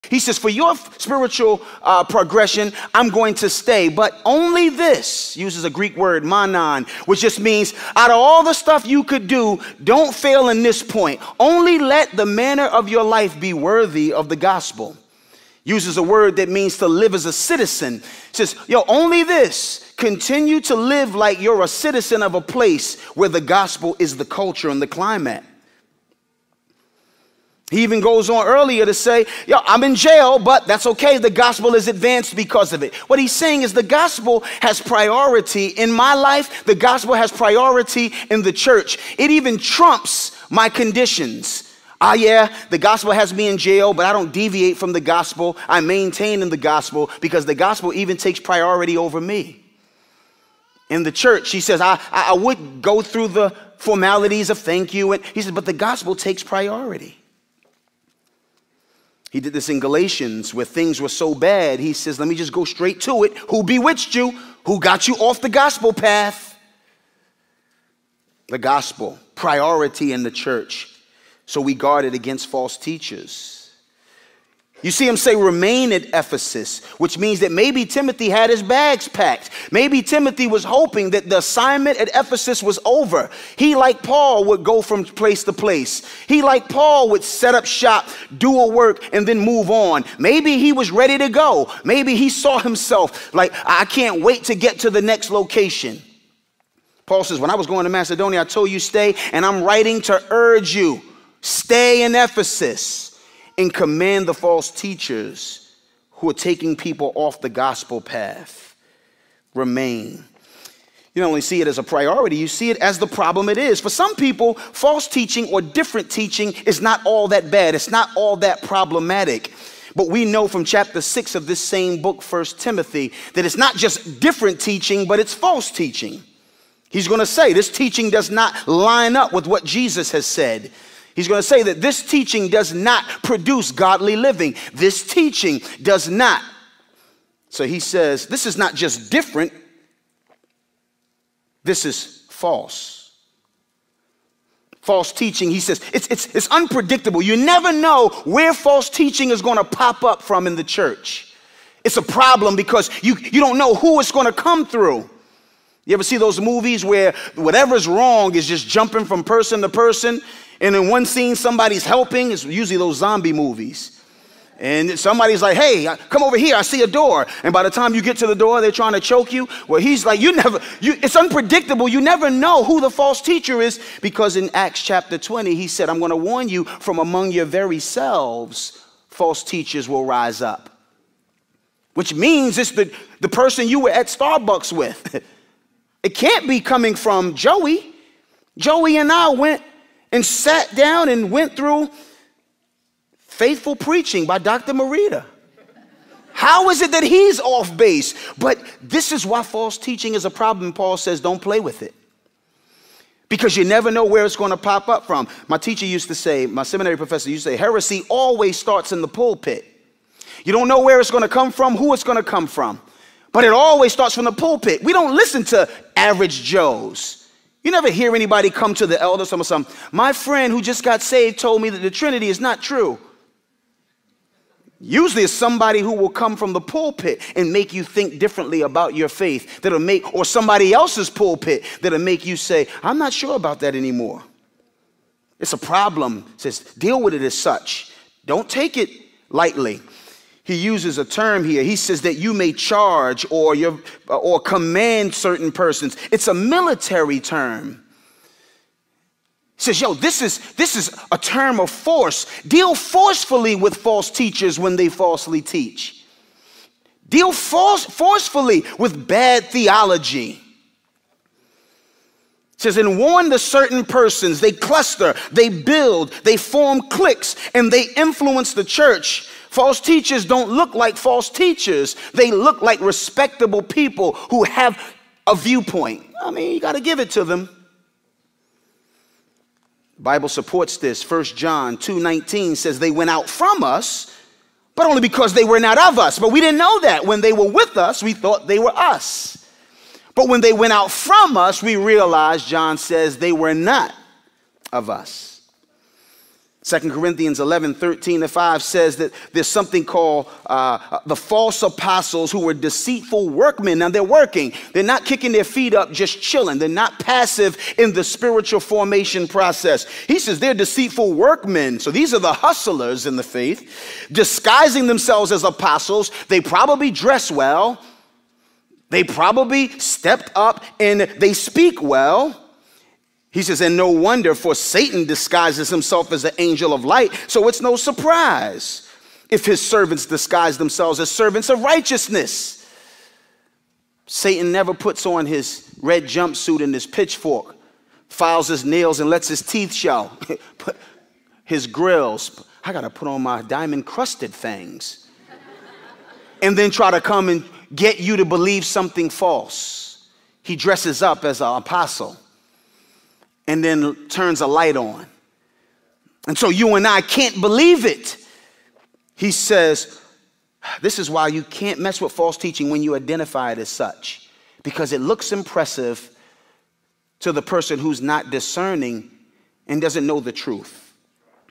William Branch- On Guard Against False Teachers
Great sermon from William Branch aka Ambassador titled “Guard The Gospel”. Hope you’re blessed by this short clip here. Full sermon below 👇🏾